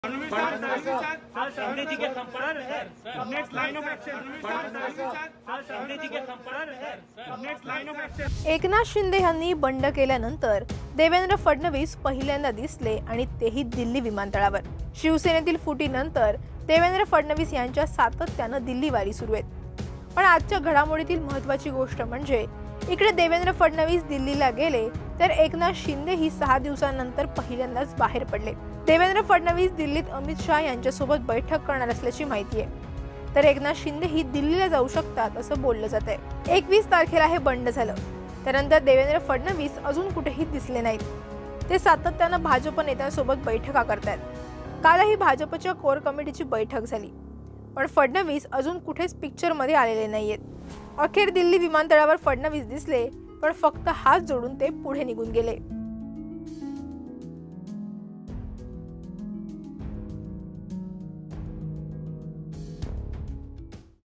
शिंदेंच्या बंडानंतर Devendra Fadnavisपहिल्यांदाच दिसले,प्रश्न विचारताच हात जोडले | Maharashtra Times
एकनाथ शिंदे यांनी बंड केल्यानंतर देवेंद्र फडणवीस पहिल्यांदाच दिसले आणि तेही दिल्ली विमानतळावर.. शिवसेनेतील फुटीनंतर देवेंद्र फडणवीस यांची सातत्याने दिल्लीवारी सुरू आहे.. पण आजच्या घडामोडीतली महत्वाची गोष्ट म्हणजे इकडे देवेंद्र फडणवीस दिल्लीला गेले, तर एकनाथ शिंदेही सहा दिवसानंतर पहिल्यांदाच बाहेर पडले.. देवेंद्र फडणवीस दिल्लीत अमित शाह यांच्यासोबत बैठक करणार असल्याची माहितीय, तर एकनाथ शिंदेही दिल्लीला जाऊ शकतात, असं बोललं जातं.. २१ तारखेला हे बंड झालं.. त्यानंतर देवेंद्र फडणवीस अजून कुठेही दिसले नाही.. ते सातत्याने भाजप नेत्यांसोबत बैठका करत आहेत. कालही भाजप कोअर कमिटीची बैठक झाली.. पण फडणवीस अजून कुठेच पिक्चरमध्ये आलेले नाहीत.. अखेर दिल्ली विमानतळावर फडणवीस दिसले.. पण फक्त हात जोडून ते पुढे निघून गेले..